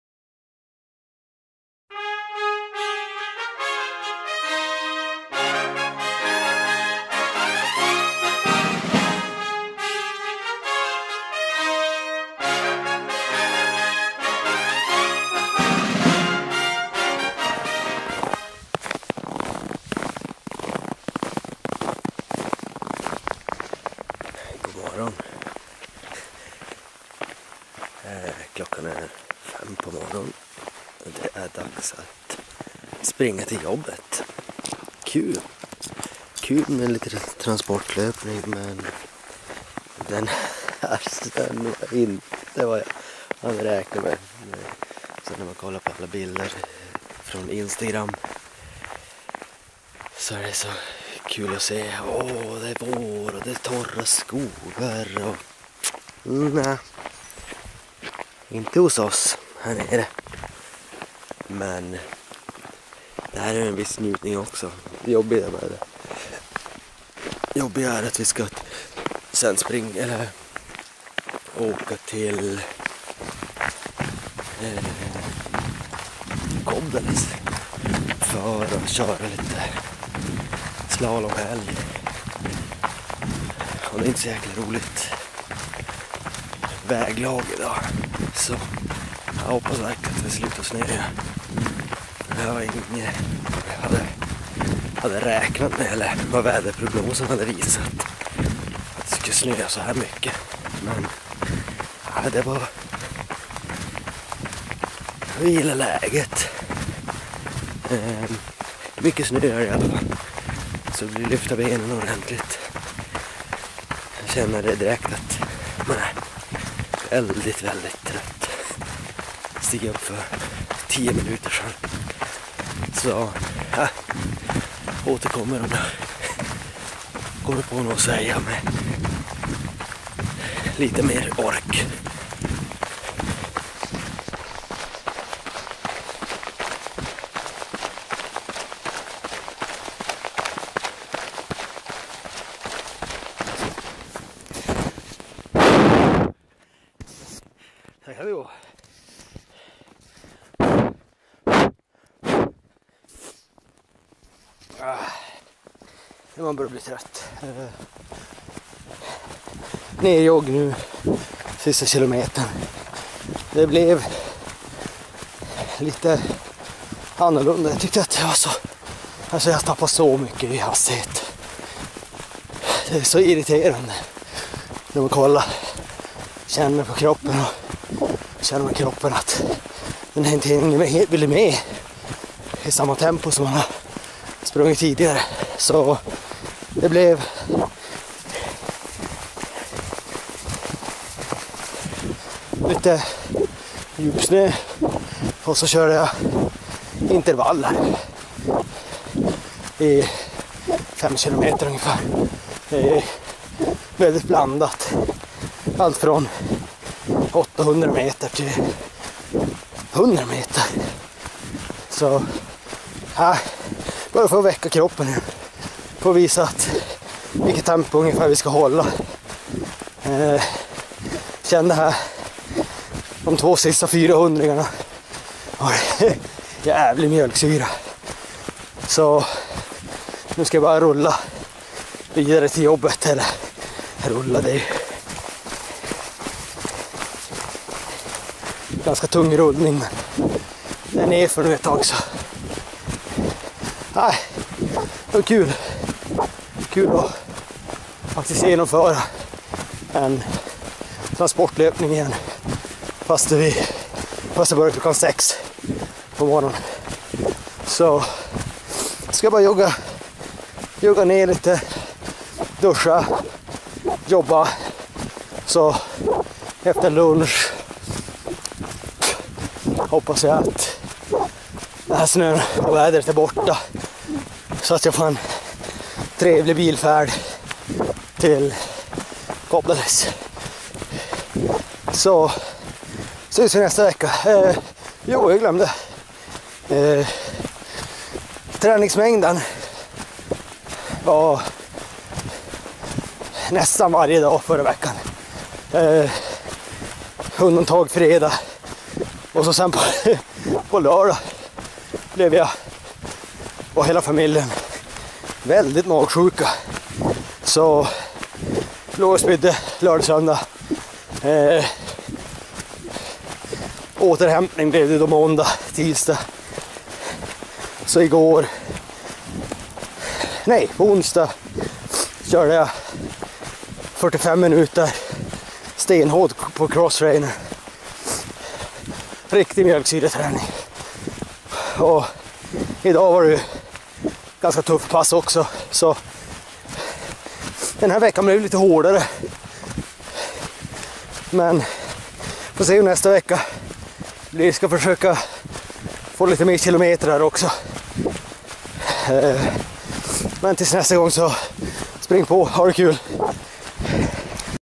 Ultralöpning 015 Transportlöpning
Transportlöpning till och från jobbet kommer bli en av nycklarna om jag skall klara av sommarens ultramaraton. Den här morgonen vräkte snön ned och de 9km till jobbet blev tyngre än väntat kl 05:00 på morgonen.
Jag tränar inför två st ultramaraton 2017, häng med från nybörjare till ultramaraton.
Vi pratar löpning i stort och ultralöpning i första hand. Träning och tankar. Jag är min egen tränare och är helt ny på det här, så kom gärna med tips och förslag